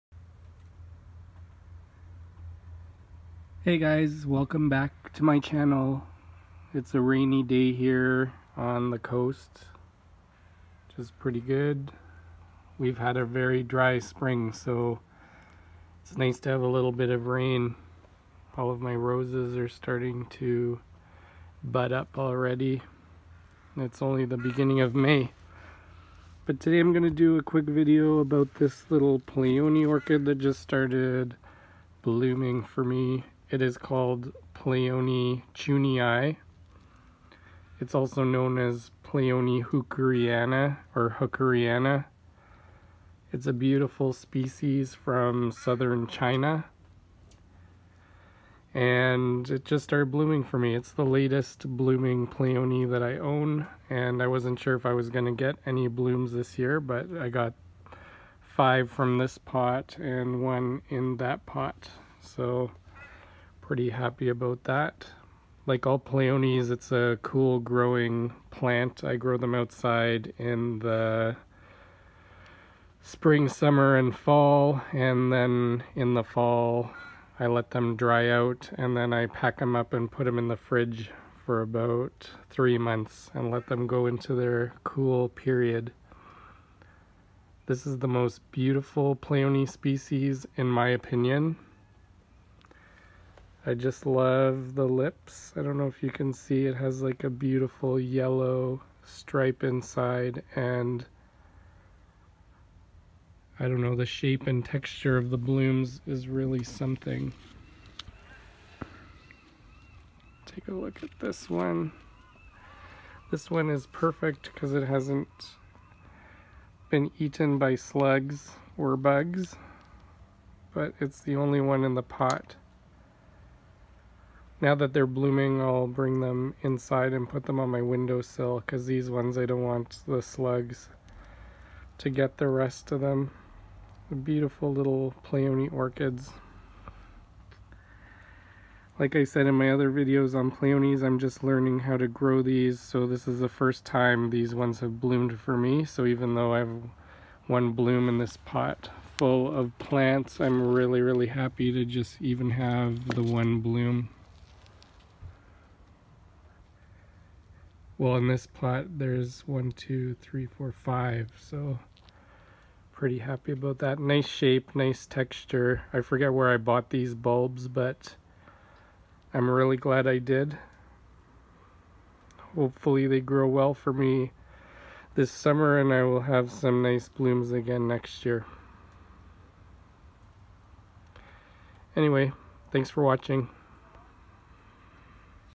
Pleione aurita Blooming :) (Not chunii as I say in the video)
Hi Guys! Sorry I mislabelled this Pleione pot - it is actually Pleione aurita! This is the first time my Pleione aurita are blooming for me :) This species is the most beautiful Pleione orchid in my opinion. Since it is starting to bloom I wanted to film it and share it with you - hope you like the blooms as much as I do!